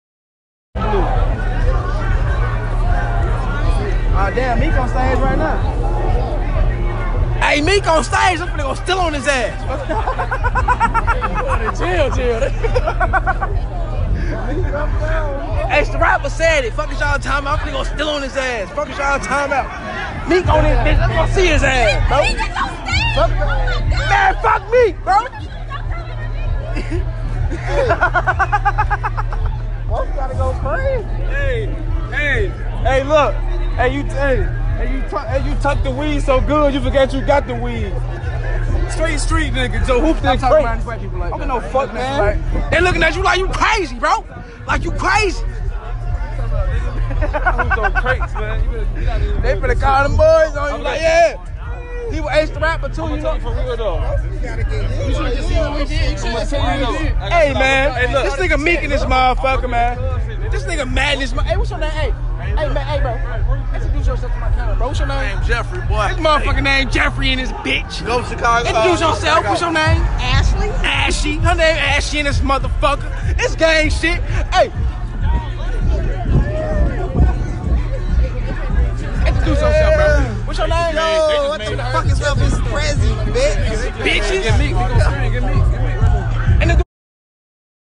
DownTown Chicago Grant Park G1 Ent Bears First game of the year Fuck Meek Mill
We Down town watching the Bears first game of the year it’s cracking as usual tune in Fuck Meek Mill G1 Ent Facebook live video Enjoy